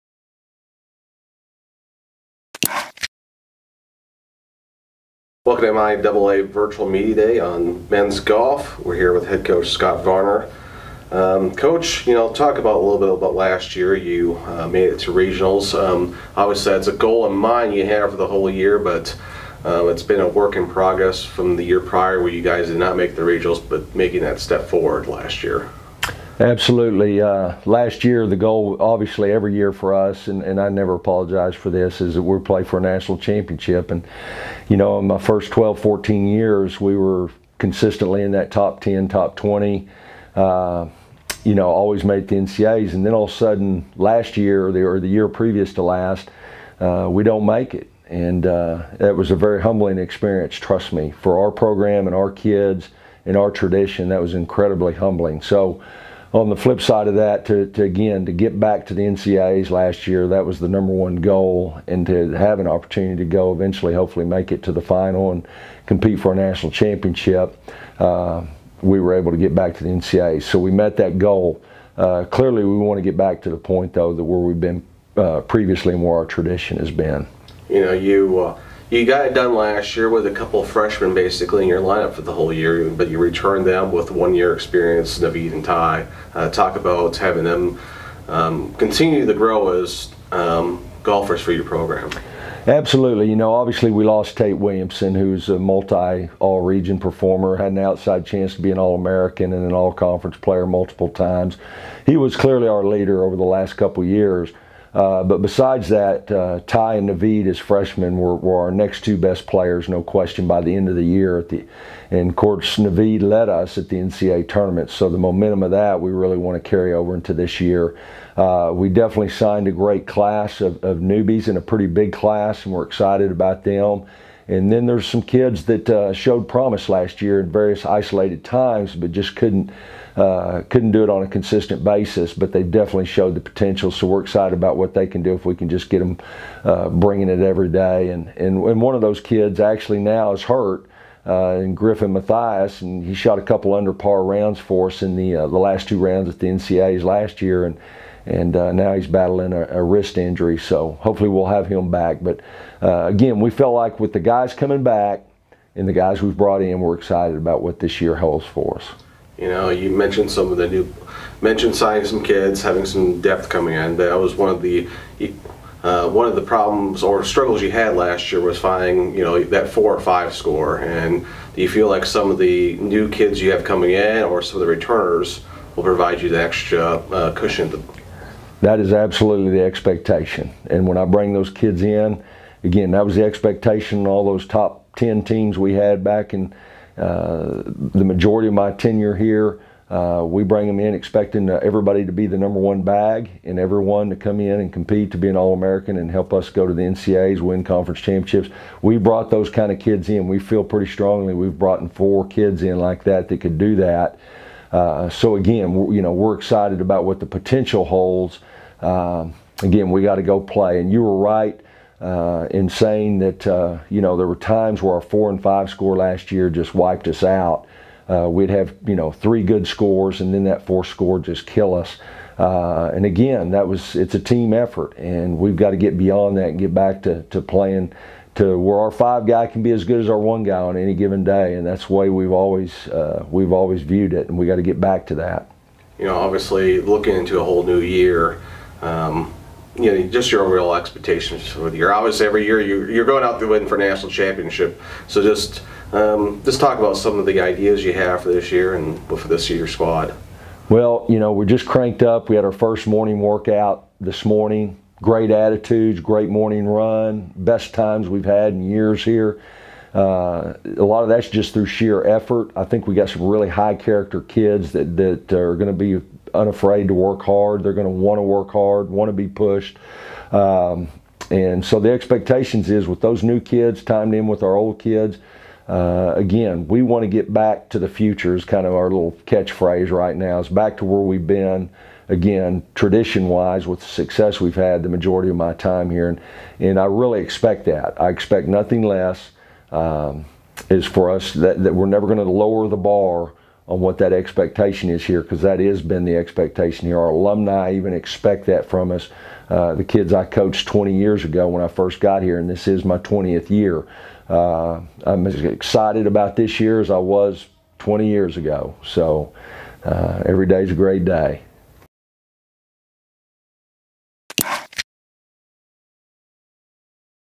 Northeastern State - MIAA Men's Golf 2017 Virtual Media Day
Head Coach Scott Varner talks about the upcoming season for the RiverHawks.